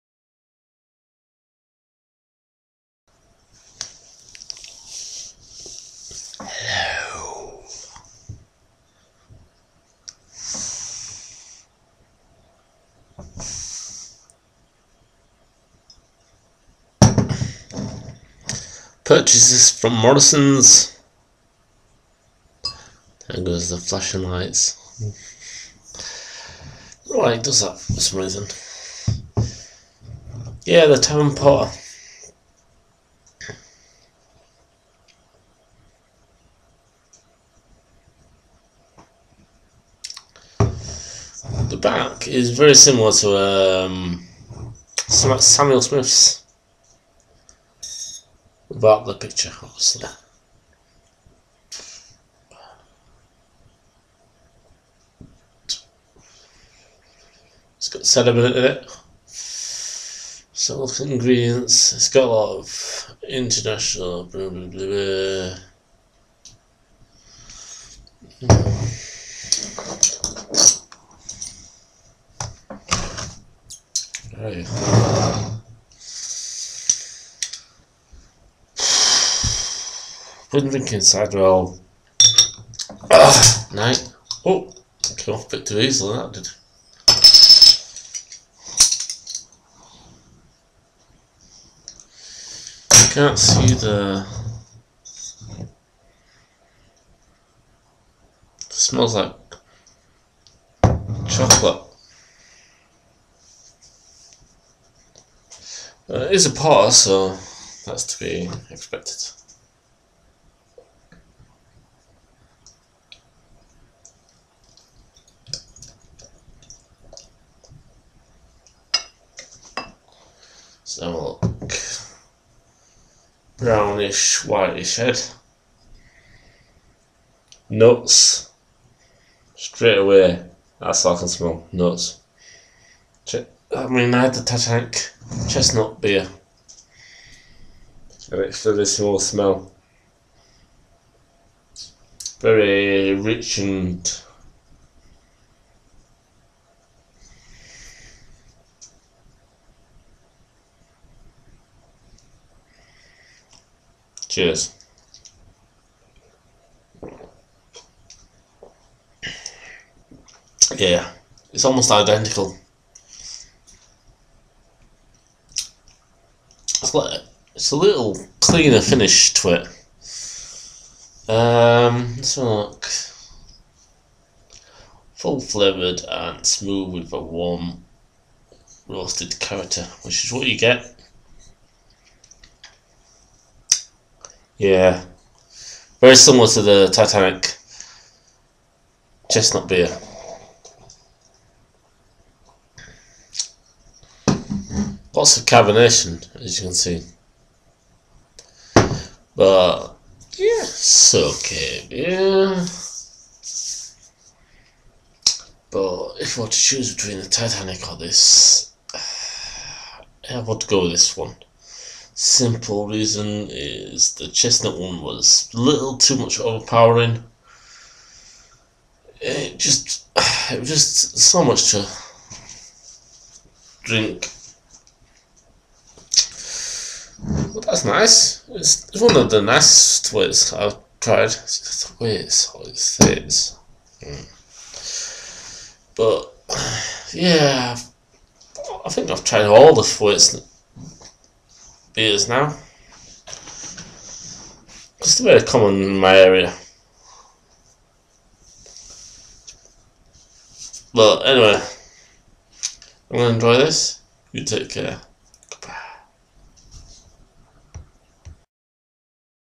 Thwaites Brewery - Tavern Porter 4.7%
Brewed by Thwaites
Style: Porter
Blackburn, England
Serve in English pint, Shaker

COMMERCIAL DESCRIPTION
Bottle Conditioned.
Surprisingly refreshing for a full-flavoured dark porter with subtle notes of smokiness and liquorice. A taste of the past, inspired by a time when the elite would gather in Elizabethan taverns, to sample the finest ales, porters and wines.